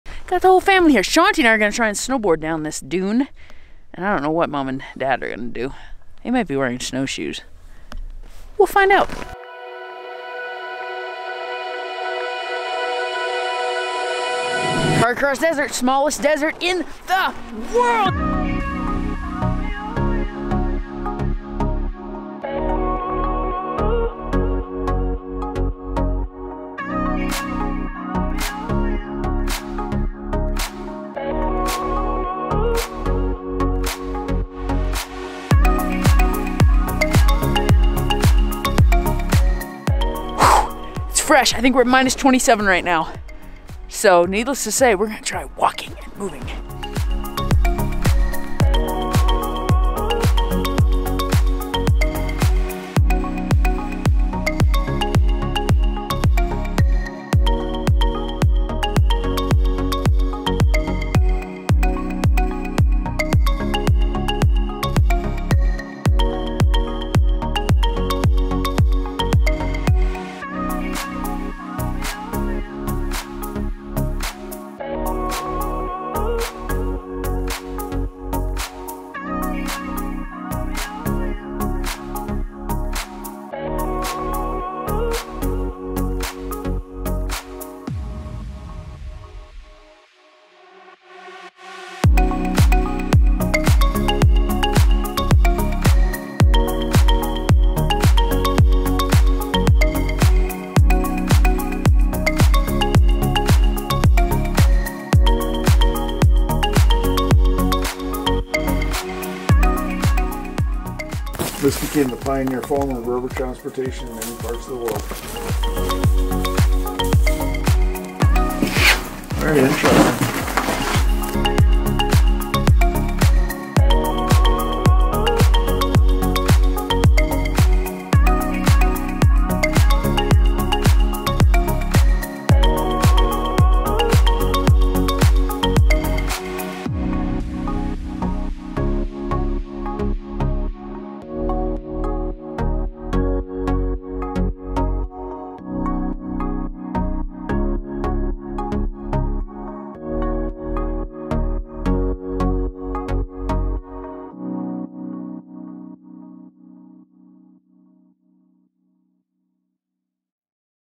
Christmas at Carcross, Yukon
Christmas In The Yukon Day #2 in Carcross, Yukon.

Today we visit what is dubbed "The Smallest Desert in the World," the Carcross Desert and did our best to snowboard down this dune. Then we made our way into the town of Carcross and took a step back in time to see the sternwheeler, the SS Tutshi.